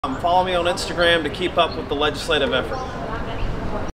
Follow Unseen Pass on Instragram for updates on Senate Bill 241
Update as of April 21, 2021:

Senate Bill 241 Passed the Senate Transportation Committee with all the members in favor!! The NCDOT is also in favor and will be following this bill closely as it keeps progressing! Their biggest concern is safety and we will be working closely with them on a few things!

This was a BIG HURDLE! Now the bill will make it to the Senate Rules Committee and then to the Senate Floor for the vote! It’s a high probably this bill is going to make it through the NC Senate and then onto the House where Larry Yarborough will most like pick it up.

Thanks @shjarvis1 for championing this bill and making this possible for NC! Without Senator Steve Jarvis, this wouldn’t be possible! Everyone, please take note!! SENATOR STEVE JARVIS is the SXS Senator!

Speaker of the House, Tim Moore
Representative Ray Pickett
Representative James Boles
Senator Vickie Sawyer
Senator Kevin Corbin
Senator David Craven
Senator Brent Jackson
Senator Julie Mayfield
Senator Dean Proctor
Senator Carl Ford
Senator Joyce Waddell
Senator Mike Woodward
Senator Todd Johnson

Thanks @quintalernie @lisahamelquintal for letting me borrow the trailer last min In Gatlinburg, TN yesterday!

Thanks to Kyle Durham, CEO at Transport Transport Company for being their, supporting me and this effort!! 💪 You can find him on FB (this is an Instagram post).

Thanks for @doug.ward.161 for the prayers!!

Thanks for Chris Brewer @chrislbrewer @brewercyclesracing @t_brewerr for supporting this effort behind the scenes and sending Justin from Team Powersports today! Thanks for @kye_laughter and Mr Perrigo @faperrigo1 for the knowledge and support! Appreciate everyone else I didn’t mention! There are so many and you know who you are.